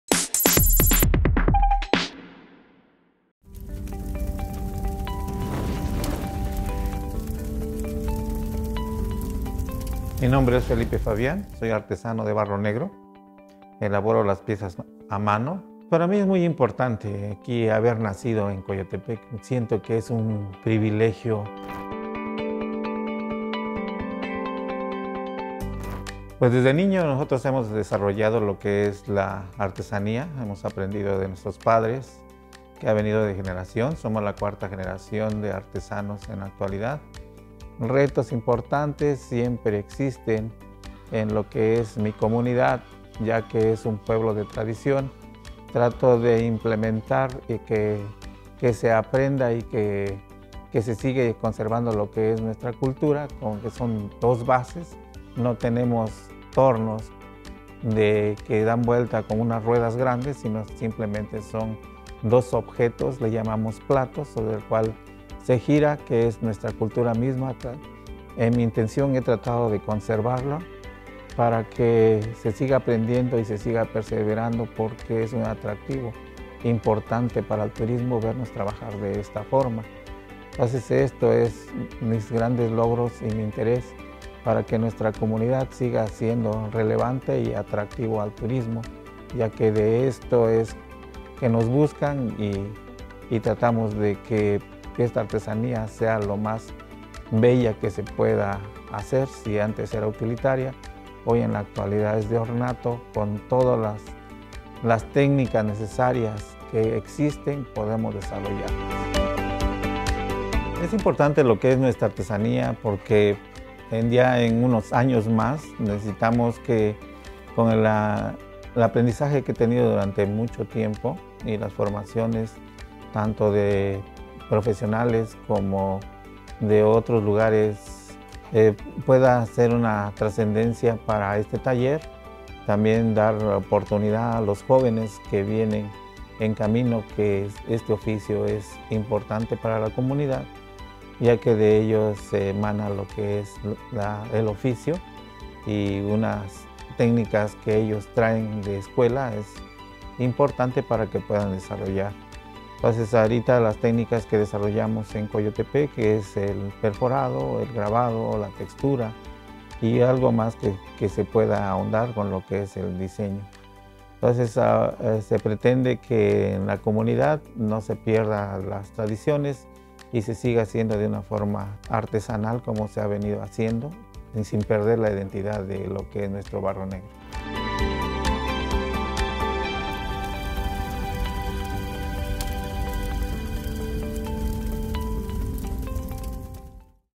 Felipe Fabián, artesano de barro negro
San Bartolo Coyotepec, en Oaxaca, se caracteriza por ser una comunidad de artesanos del barro negro